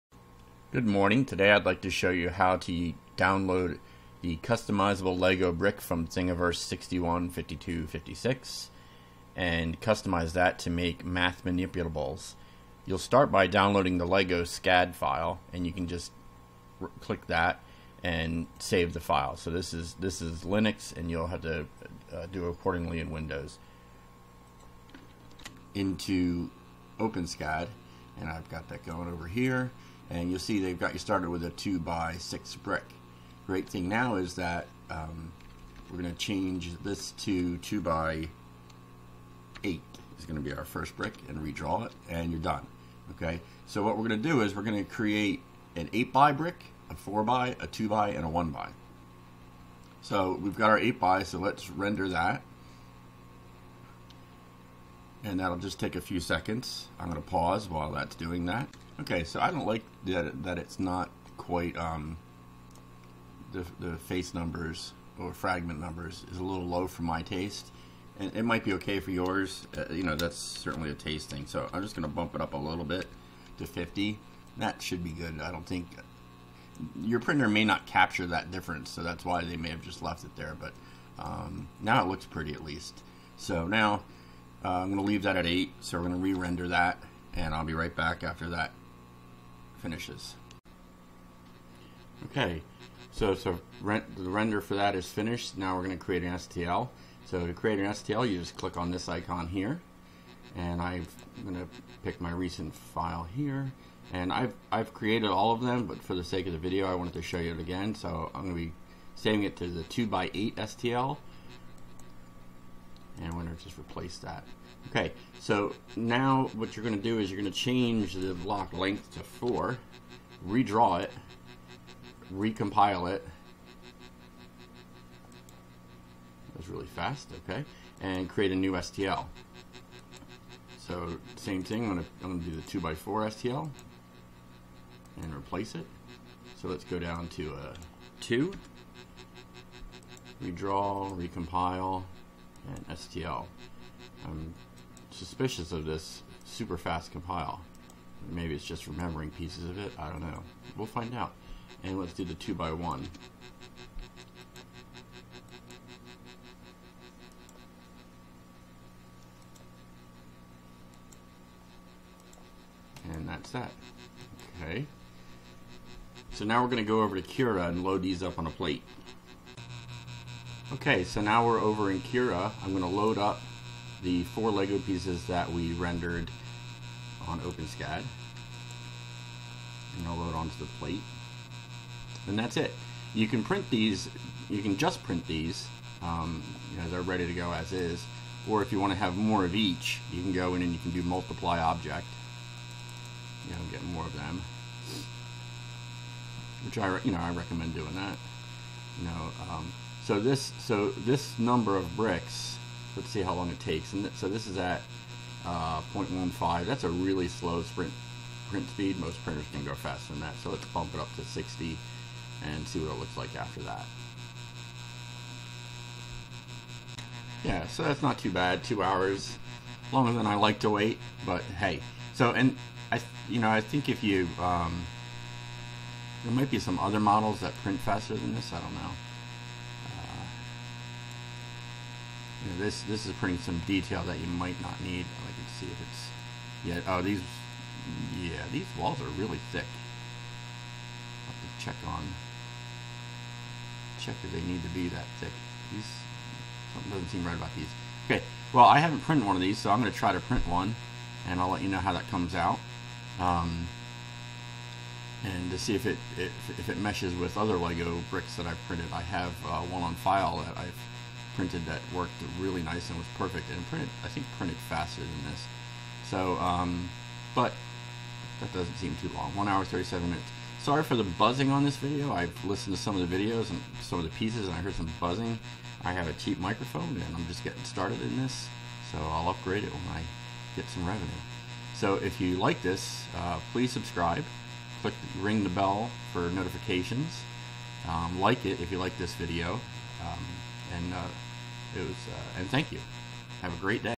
OpenSCAD - Making Legos4Math from Thingiverse Legos
Using a customizable lego from thingiverse I show you how to customize it and load it onto Cura.

This is a very basic demo, if you already use a 3d printer you probably don't need to watch this. :)